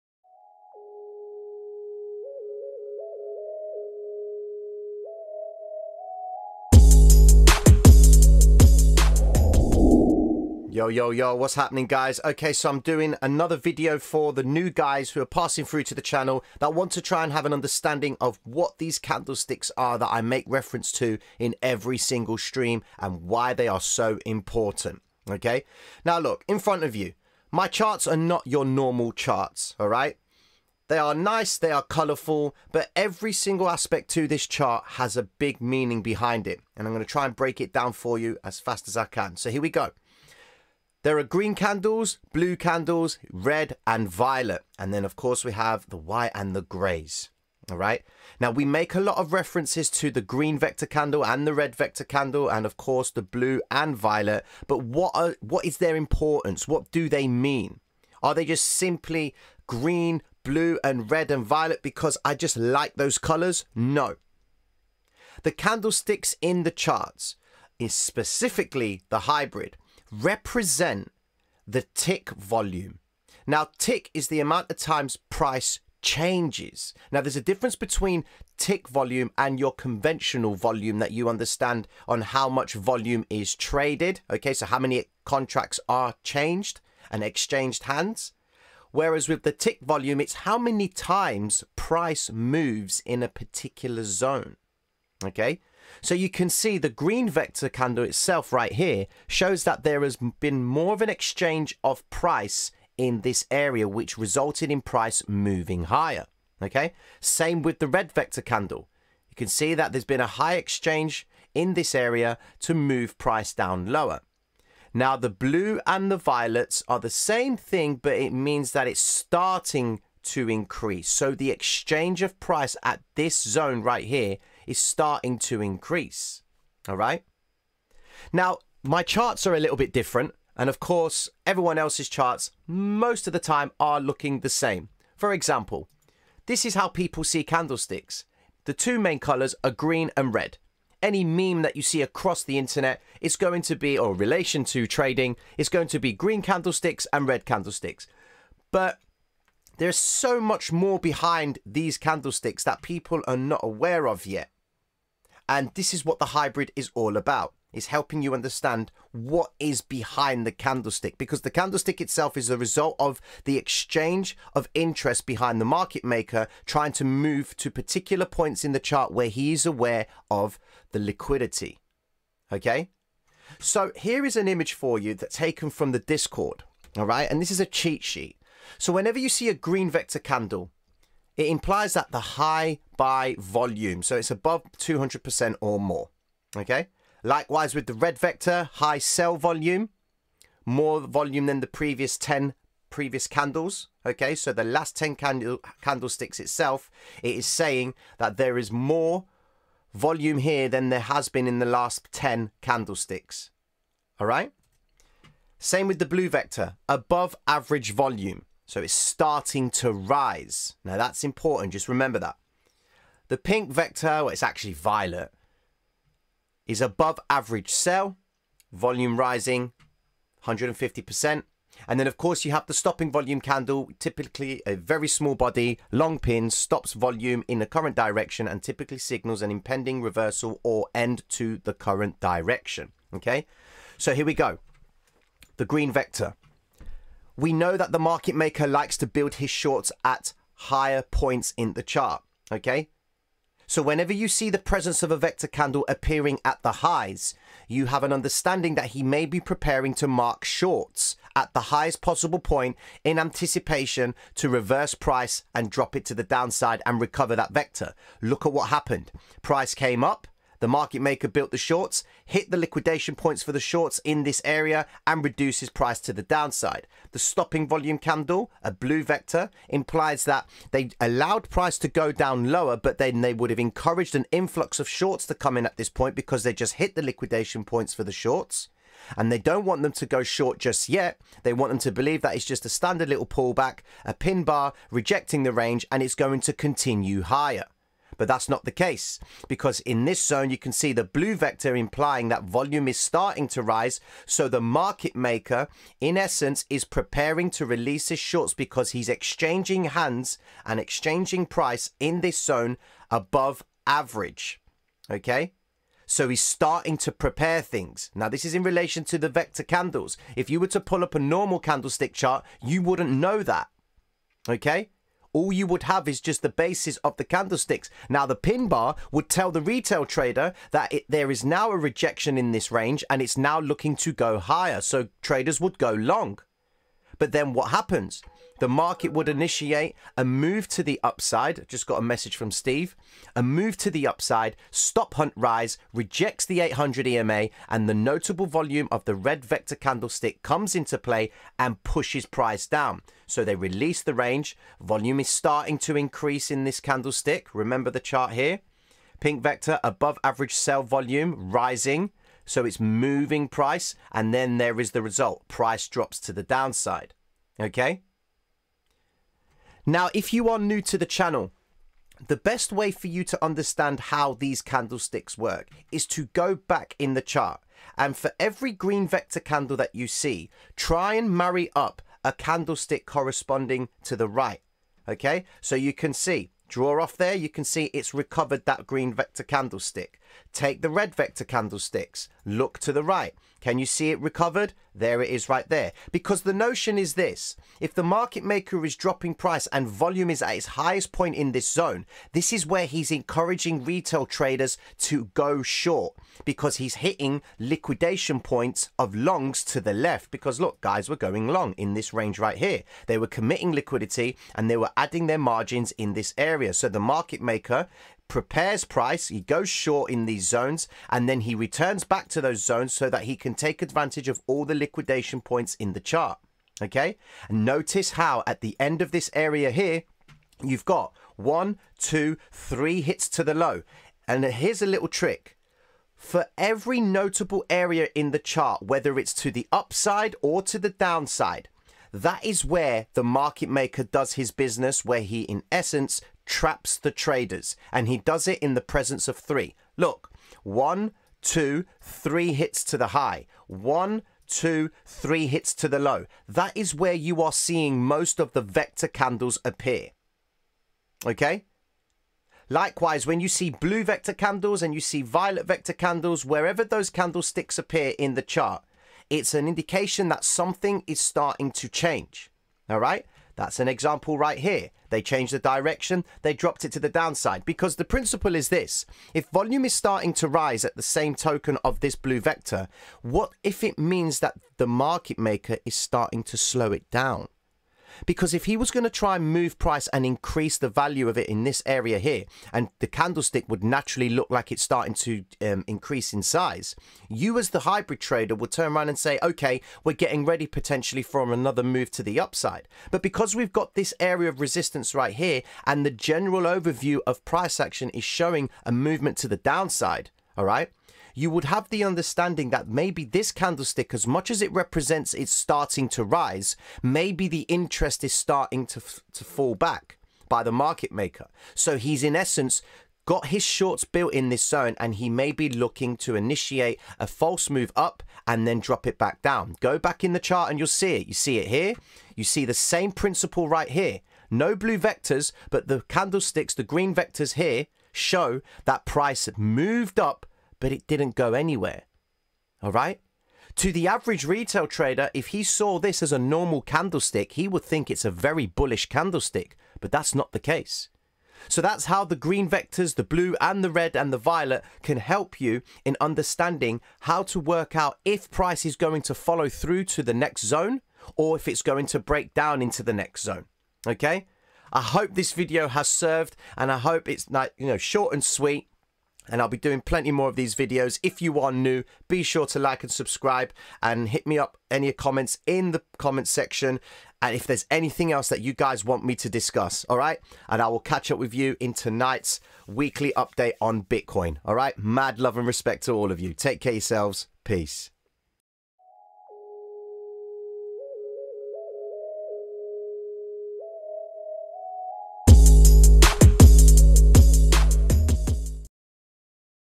How To Read The Hybrid Candlesticks (Trading Bitcoin)
Yo, guys, this is a quick video on how to read candlesticks when trading bitcoin. Bitcoin today appears to be attempting to break down, now this video will help you understand what the candlesticks are trying to show you in relation to the market makers' intentions regarding moving towards previous zones of liquidity.

Traders Reality Ultimate Guide To Trading Psychology